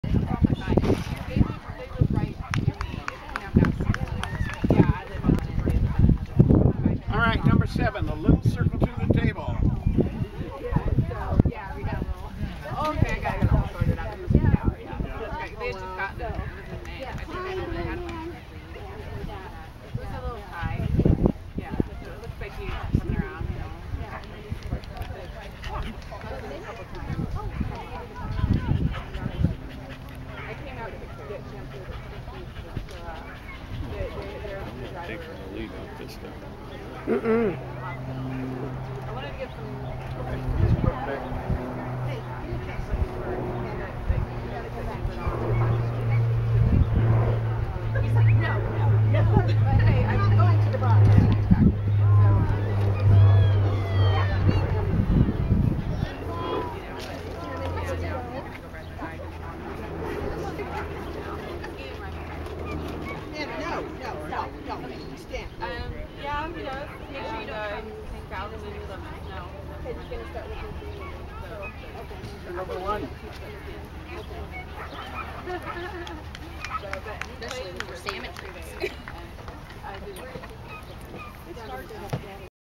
Cisco's First Show
At 6 months old, Hadacall Merrily's The Cisco Kid showed at the first day of the Mt Bachelor Kennel Clubs show in Redmond, Oregon. I was very proud of him!